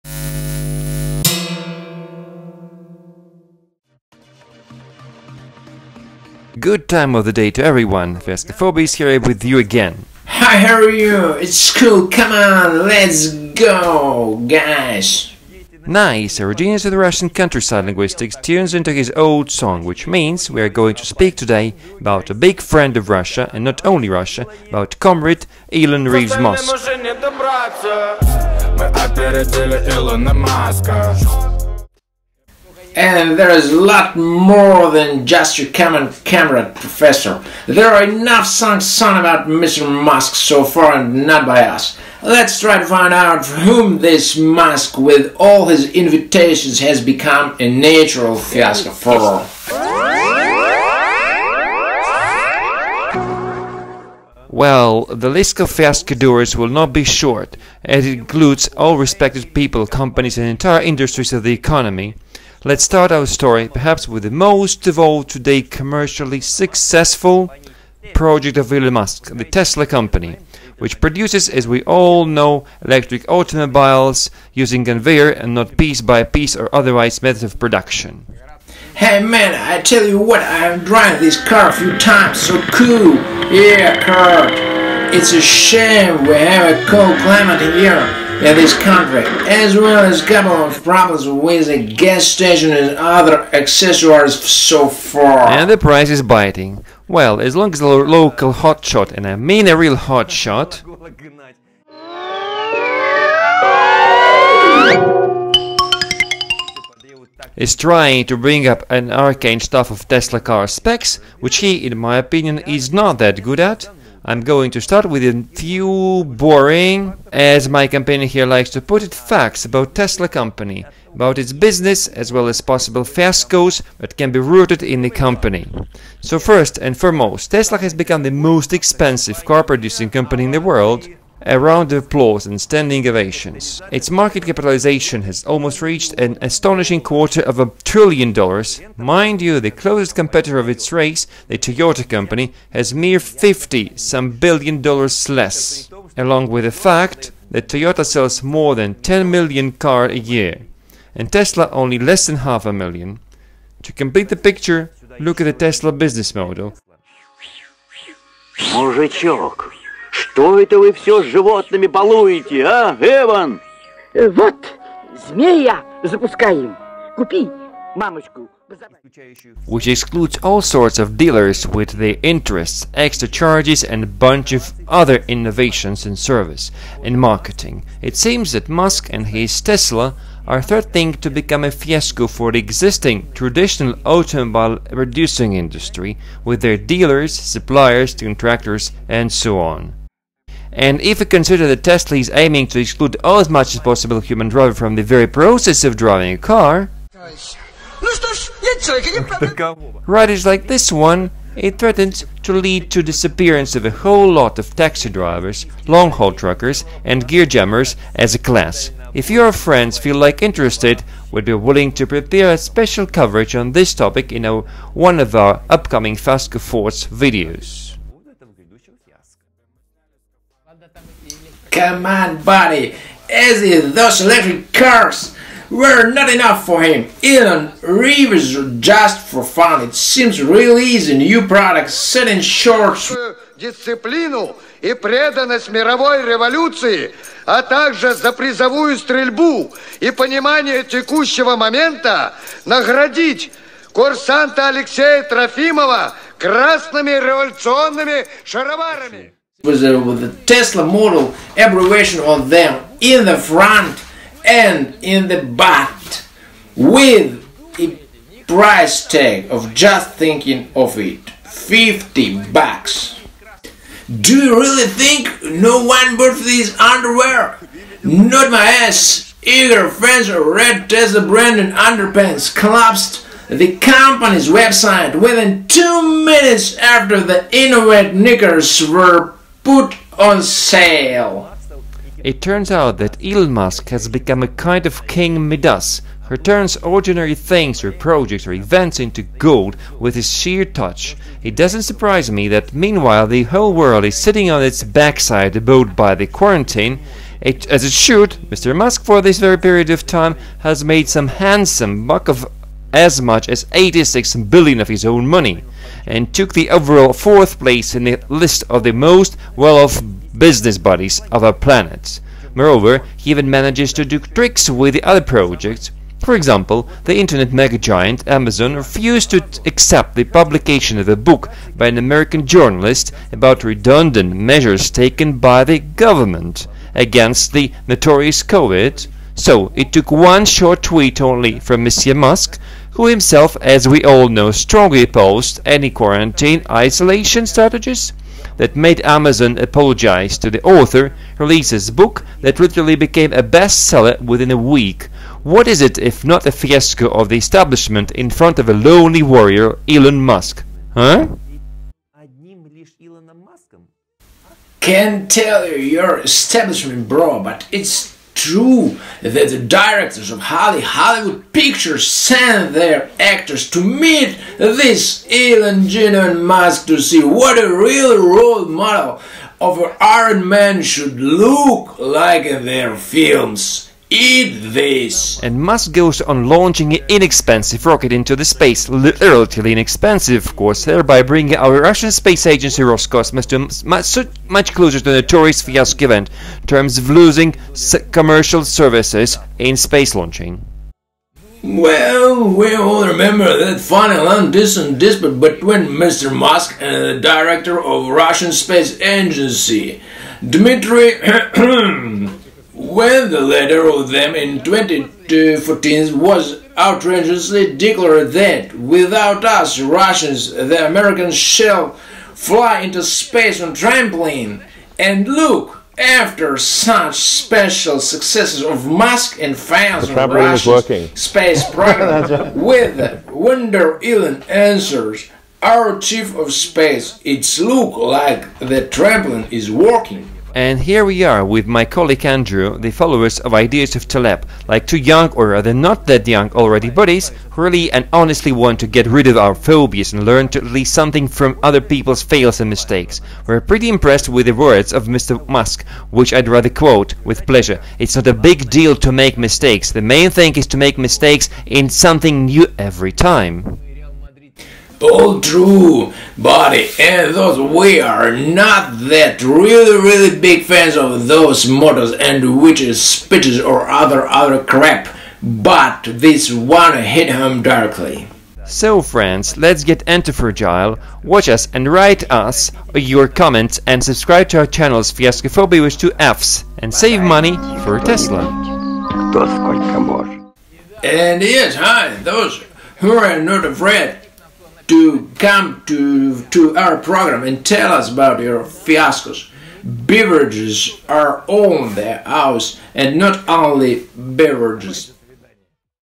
Fiasco Fobia.A Stun Gun for the Automobile Industry
Hello, dear ladies and gentlemen FiascoFobiacs! Today we decided to offer you a topic for discussion that is extremely technological and even futuristic in some places. As they say, the future enters our lives under the subtle hum of an electric motor! Let's talk about Elon Reeves Musk and his body shop called "Tesla, Inc.". So many spears have already been broken about how adventurous, unprofitable and technically unworkable Mr. Musk's projects are, that we have nothing to add about this. But what we would be much more interested in finding out is maybe it's not Musks who's rushing in his Tesla in fear into the abyss in a bottomless Fiasco? Maybe it's us, who are now laughing at him and his ideas, slowly but inexorably being implemented in practice, maybe we are, figuratively speaking, playing for a team of hopeless losers..? Share your opinion, gentlemen, electric motorists and not only! We expect your thoughts, faithfully yours, FiascoFobia. And let our  batteries never run flat!